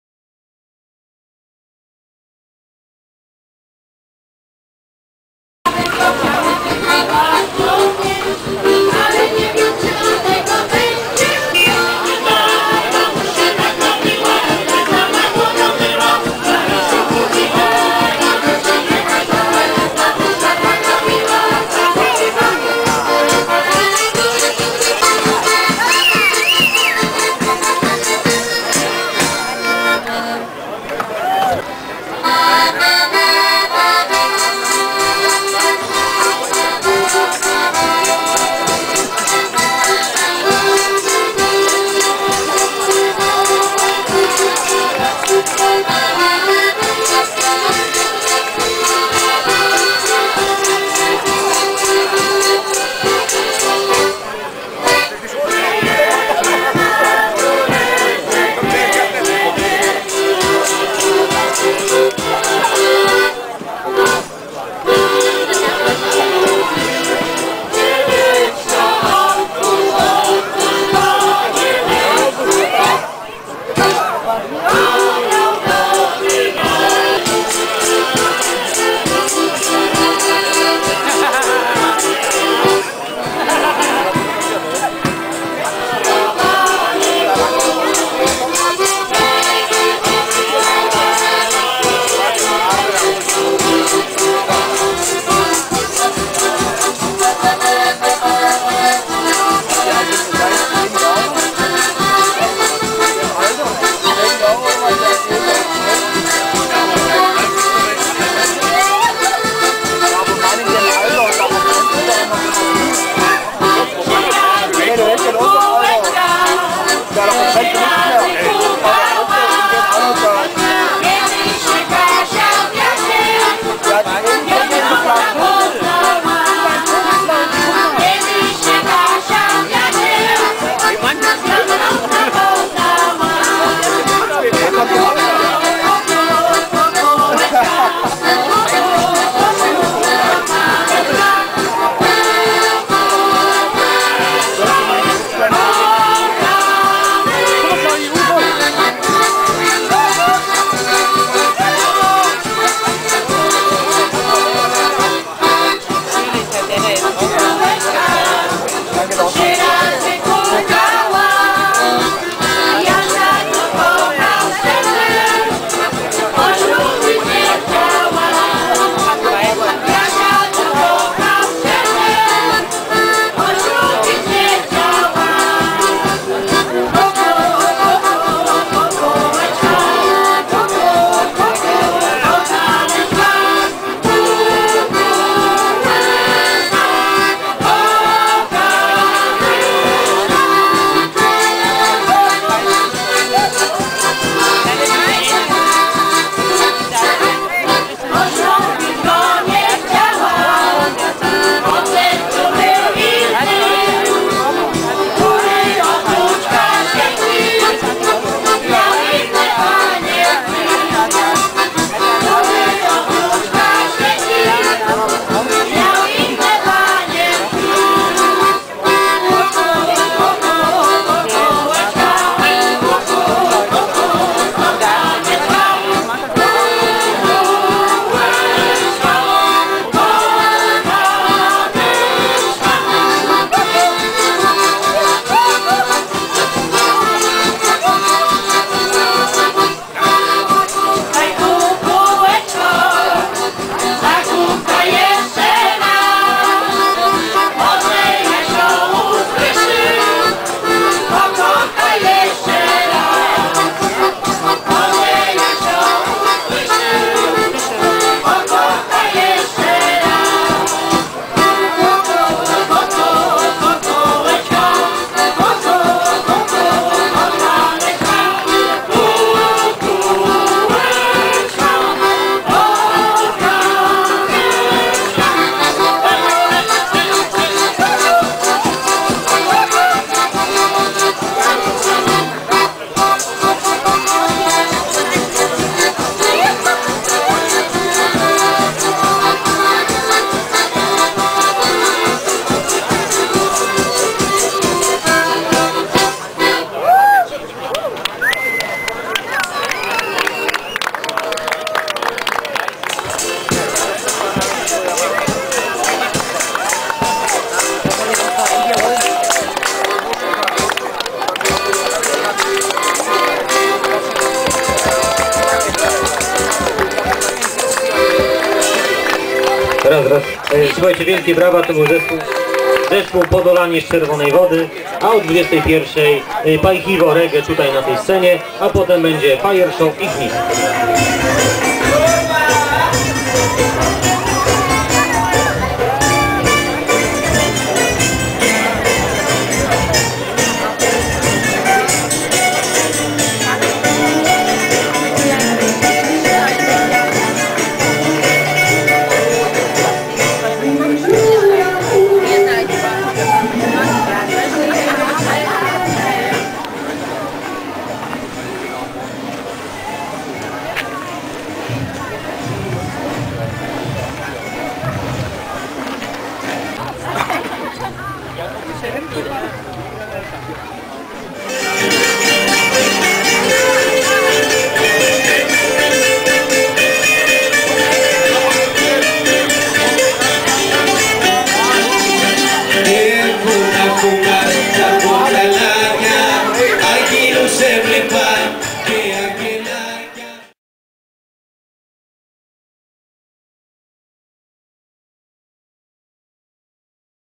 18. Folklorum auf der Kulturinsel Einsiedel bei Görlitz 2011
Das 18. Folklorum hat erstmal die polnischen Neißewiesen in das Festivalgelände einbezogen. Die hölzerne Neißefähre wurde zum wahren "Wanderbrückum". Das Folklorum ist zugleich leistungsschau der Firma Künstlerische Holzgestaltung Jürgen Bergmann und zieht jährlich mehr als zehntausend Besucher an, "2einhalb Tage anders sein" zu erleben - und eines der schönsten Folkfestivals überhaupt. Im Herzen der Zentral-Lausitz!